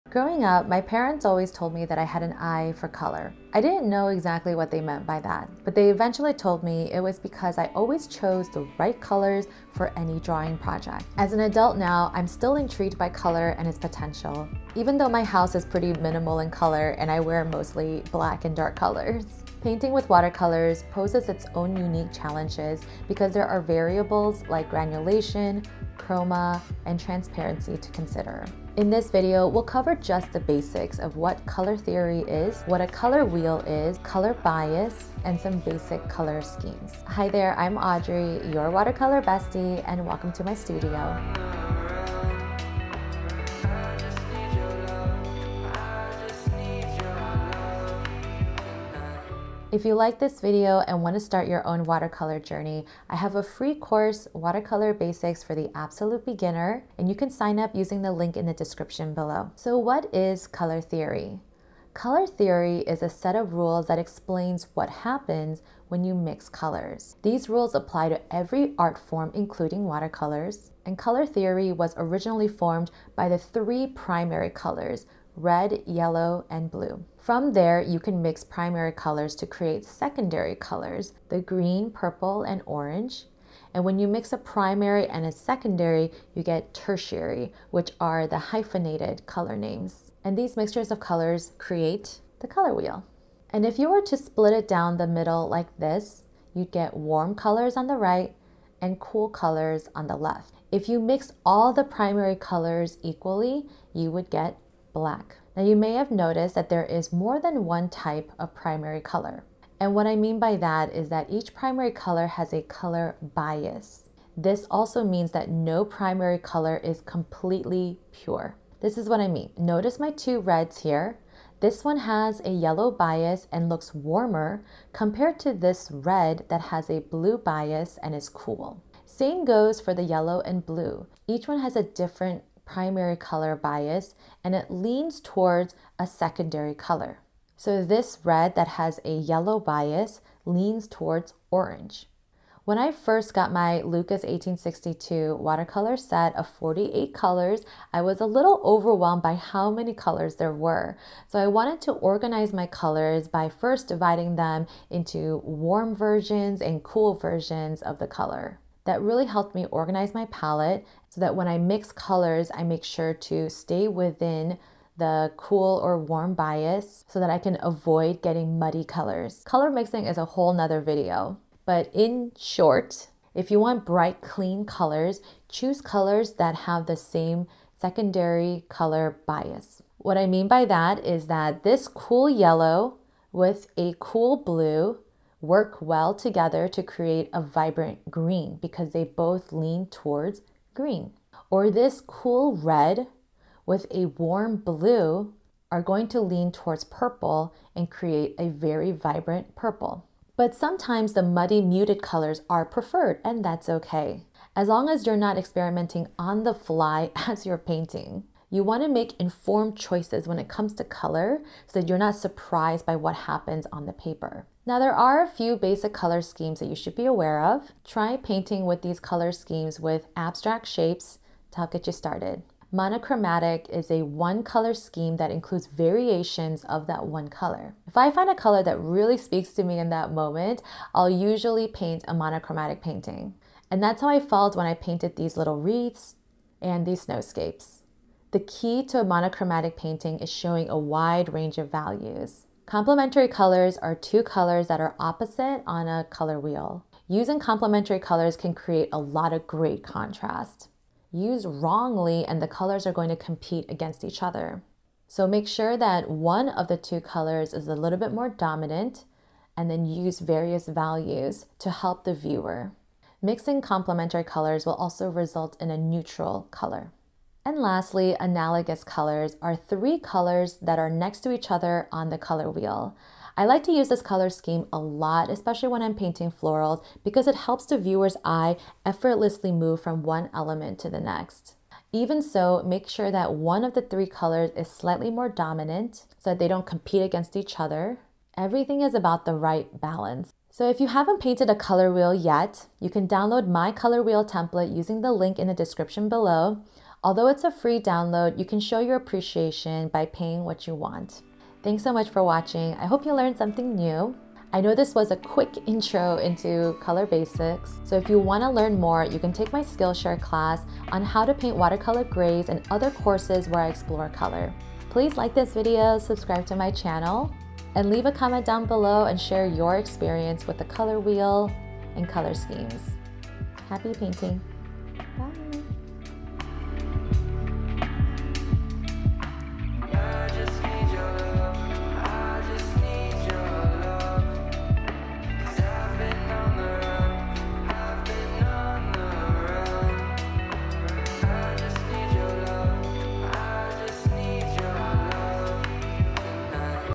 Watercolor Color Theory, Color Wheel, and other Color Basics!
Color theory is so important with watercolors! In this video I discuss the importance of color bias, what a color wheel is, and some color schemes that you should try in your next painting.

Download the free color wheel templates (there are three of them!)

If you enjoyed this vlog, buy me a ☕️ so I can keep creating!

All supplies can be found at Jerry's Artarama! ​ @JerrysArtarama

More videos about color!